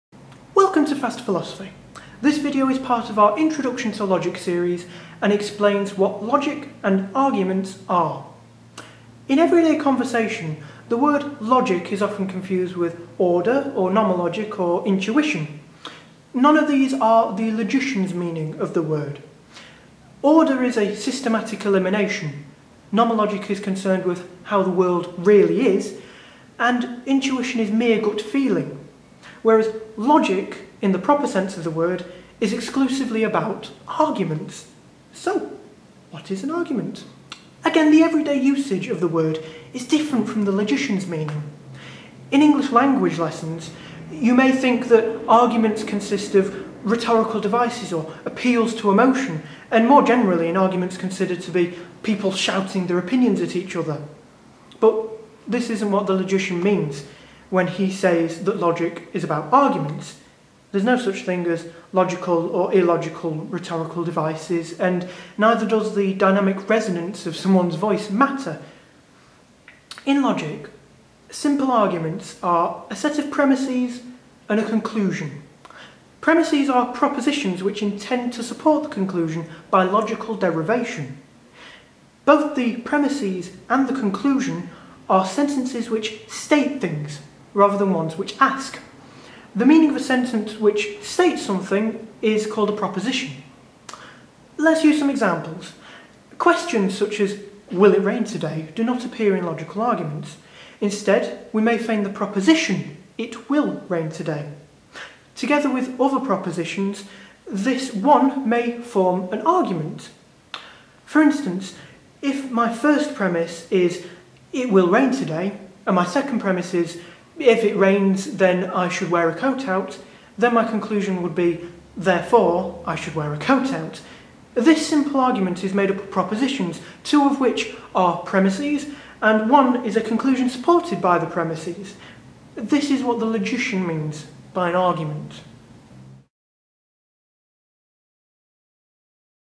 01x01 - What Logic And Arguments Are
Welcome to Fast Philosophy. This video is part of our Introduction To Logic series and explains what logic and arguments are.
In everyday conversation, logic is often confused with order, nomologic or intuition. None of these are the logician's meaning of the word. Order is a systematic elimination, nomologic is concerned with how the world really is and intuition is mere gut feeling, whereas logic in the proper sense of the word is exclusively about arguments.
So what is an argument? Again, the everyday usage of the word is different from the logician's meaning. In English Language, an argument may consist of rhetorical devices such as appeals to emotion or questions; and more generally, an argument is thought to be people shouting their opinions. But the logician means something different when he says that logic is about arguments: there is no such thing as a logical or illogical question, and neither does the dynamic resonance of someone's voice matter.
In logic, simple arguments are a set of premises and a conclusion. Premises are propositions which intend to support the conclusion by logical derivation. Both the premises and the conclusion are sentences which state things rather than ones which ask. The meaning of a sentence which states something is called a proposition.
Let's use some examples. Questions such as 'Will it rain today?' do not appear in logical arguments. Instead, we may find a proposition such as 'It will rain today'. Together with other propositions, this one may form an argument. For instance, if my first premise is 'It will rain today' and my second premise is 'If it rains today, then I should wear a coat out', then my conclusion would be 'Therefore, I should wear a coat out'. This is a simple argument made up of propositions -- two of which are premises, and the other is a conclusion supported by the premises. This is what the logician means by an argument.